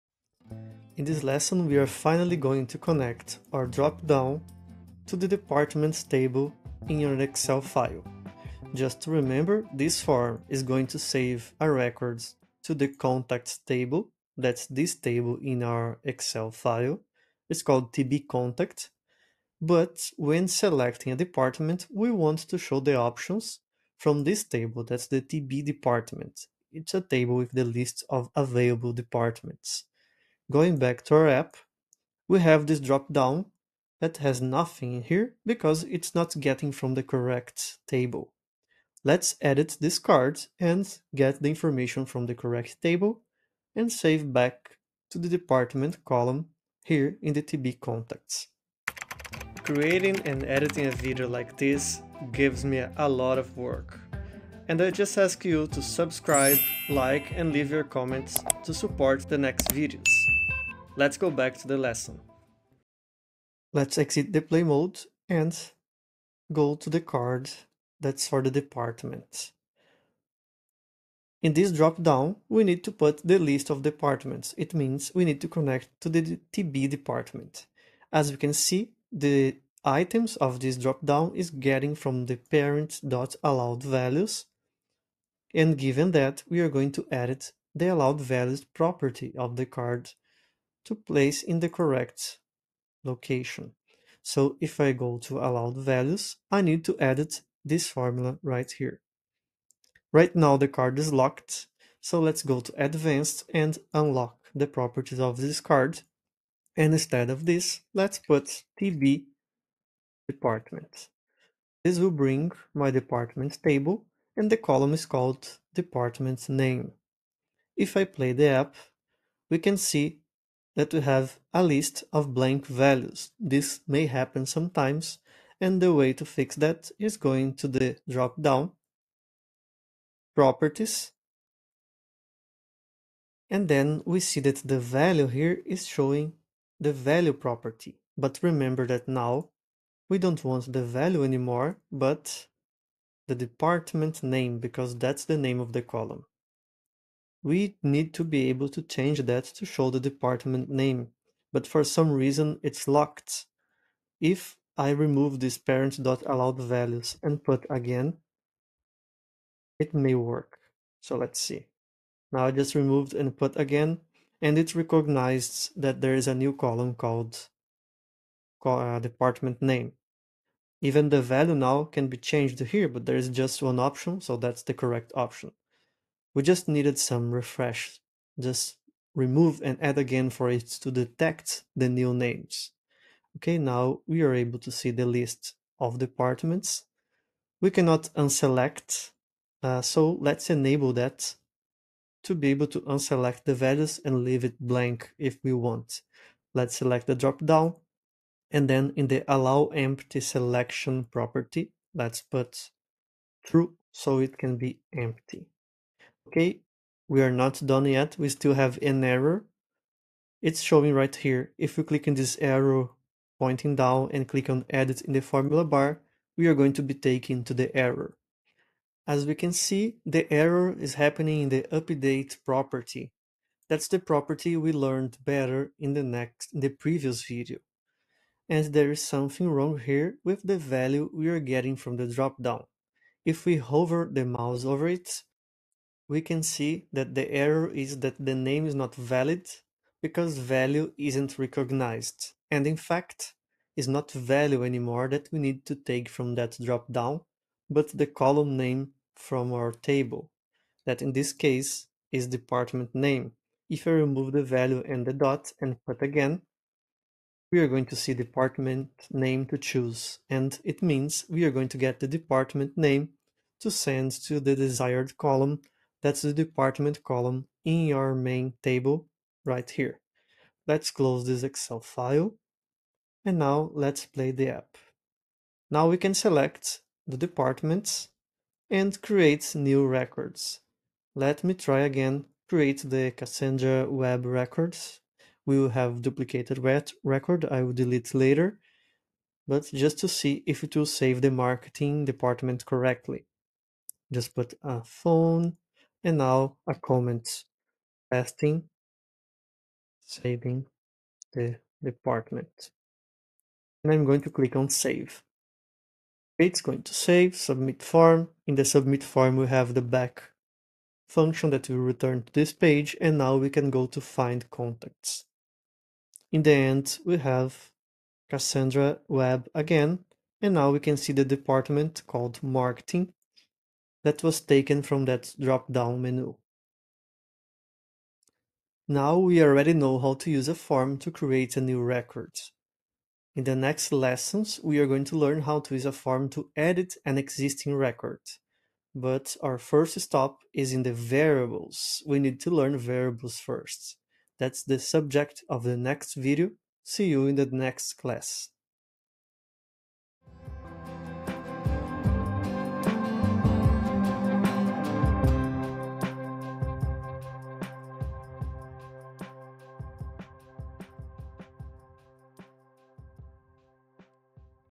Connecting your Power Apps form dropdown to a different table (Excel) - Power Apps
In this Power Apps tutorial for beginners, we'll learn how to connect a dropdown menu to the departments table in Excel. Our goal is to ensure that when users select a department, they can choose from options listed in the TB department table, while saving records to the TB contact table.

Follow along as we navigate through the Power Apps interface to edit the dropdown card and adjust its properties. We'll unlock the card, modify the allowed values, and ensure that the dropdown retrieves and displays the correct department names.

Throughout the tutorial, we'll encounter common issues such as blank dropdown values and errors in the Update property. We'll troubleshoot these challenges step by step, ensuring that our dropdown functions smoothly and accurately.

By the end of this tutorial, you'll have a solid understanding of how to connect dropdown menus to data tables in Power Apps, empowering you to create dynamic and user-friendly forms for your applications.

Key Learning Points:

- Configuring dropdowns to fetch data from specific tables in Excel.
- Editing properties within Power Apps to accurately display and submit data.
- Troubleshooting common issues with dropdown data linkage in Power Apps.
- Enabling the option to leave dropdown selections unselected for user flexibility.
- Correcting data submission errors by properly linking dropdown selections to the correct database column.

Don't miss out on mastering this essential skill in Power Apps development! Watch the tutorial now and level up your app-building expertise.